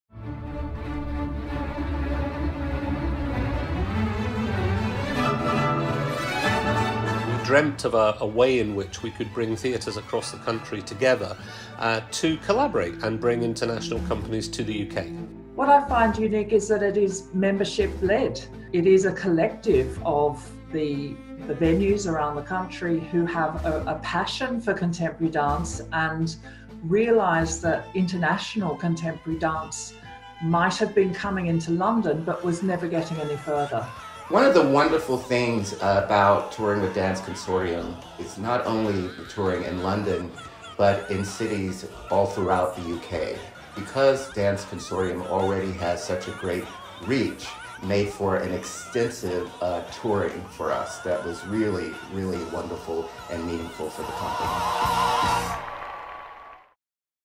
Dance Consortium | 20th Anniversary film | Working as a Consortium
As part of Dance Consortium's 20th anniversary we have made a short film that celebrates the brilliant artists and companies that we have had the pleasure to work with.

Since 2000, the 18 UK member theatres that make up Dance Consortium have toured the work of international artists and companies to audiences across the UK,  presenting 46 tours by 26 companies to over 700,000 people.  From Cuba to the Netherlands, Brazil to America, Australia to South Africa and Canada to China our aim has been to connect local audiences to the best contemporary dance companies from across the globe.

Thanks to all of those that have shared their memories and reflections, we hope you enjoy the film.